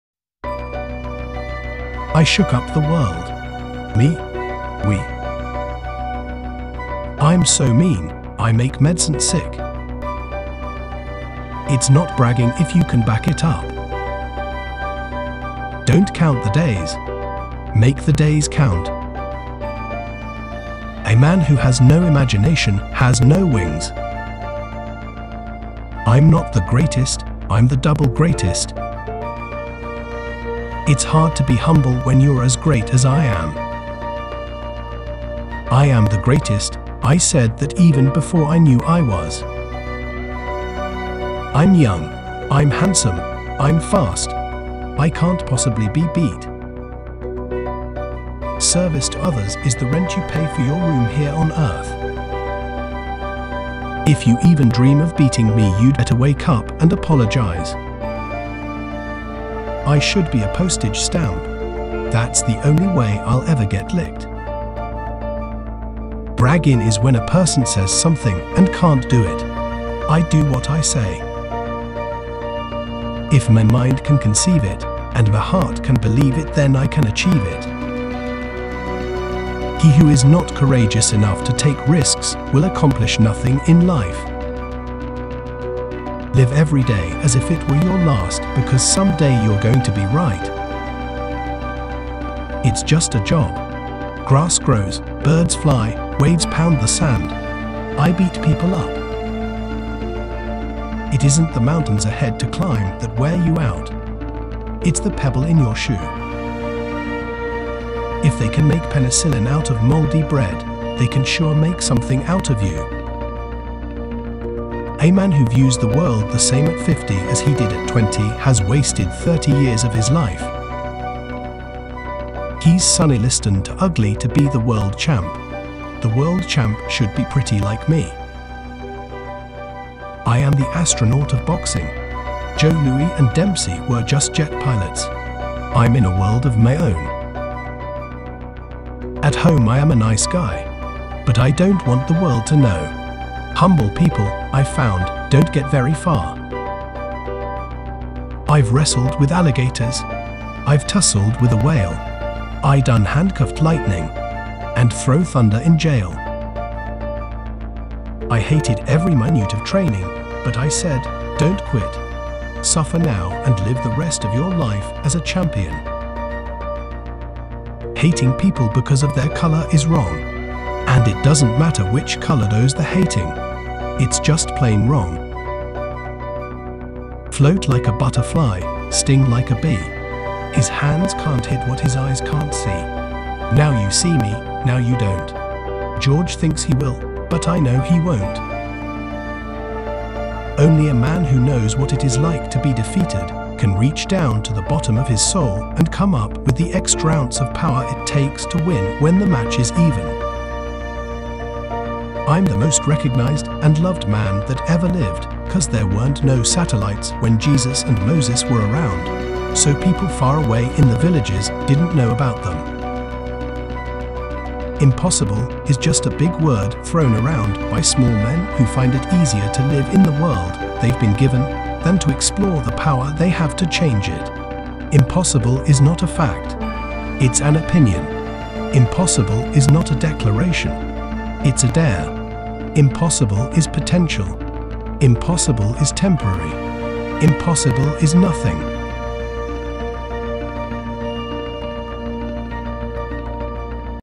The Greatest - Muhammad Ali Best Inspirational Quotes | Quotes From Legends
Muhammad Ali Best Inspirational Quotes:

"I shook up the world. Me! Whee!"

"I'm so mean, I make medicine sick."

“It’s not bragging if you can back it up.”

"Don’t count the days; make the days count."

"A man who has no imagination has no wings."

“I’m not the greatest, I’m the double greatest.”

“It's hard to be humble when you're as great as I am."

"I am the greatest, I said that even before I knew I was."

"I'm young; I'm handsome; I'm fast. I can't possibly be beat."

"Service to others is the rent you pay for your room here on earth."

“If you even dream of beating me you'd better wake up and apologize.”

"I should be a postage stamp. That's the only way I'll ever get licked."

“Braggin' is when a person says something and can’t do it. I do what I say.”

“If my mind can conceive it, and my heart can believe it—then I can achieve it."

“He who is not courageous enough to take risks will accomplish nothing in life.”

“Live everyday as if it were your last because someday you're going to be right.”

"It's just a job. Grass grows, birds fly, waves pound the sand. I beat people up."

“It isn’t the mountains ahead to climb that wear you out; it’s the pebble in your shoe.”

“If they can make penicillin out of moldy bread, they can sure make something out of you.”

"A man who views the world the same at 50 as he did at 20 has wasted 30 years of his life.”

"He’s (Sonny Liston) too ugly to be the world champ. The world champ should be pretty like me!"

"I am the astronaut of boxing. Joe Louis and Dempsey were just jet pilots. I'm in a world of my own."

“At home I am a nice guy: but I don’t want the world to know. Humble people, I’ve found, don’t get very far.”

“I’ve wrestled with alligators. I’ve tussled with a whale. I done handcuffed lightning. And throw thunder in jail.”

“I hated every minute of training, but I said, ‘Don’t quit. Suffer now and live the rest of your life as a champion.'”

"Hating people because of their color is wrong. And it doesn't matter which color does the hating. It's just plain wrong."

"Float like a butterfly, sting like a bee. His hands can't hit what his eyes can't see. Now you see me, now you don't. George thinks he will, but I know he won't."

"Only a man who knows what it is like to be defeated can reach down to the bottom of his soul and come up with the extra ounce of power it takes to win when the match is even."

"I'm the most recognized and loved man that ever lived cuz there weren't no satellites when Jesus and Moses were around, so people far away in the villages didn't know about them."

“Impossible is just a big word thrown around by small men who find it easier to live in the world they’ve been given than to explore the power they have to change it. Impossible is not a fact. It’s an opinion. Impossible is not a declaration. It’s a dare. Impossible is potential. Impossible is temporary. Impossible is nothing.”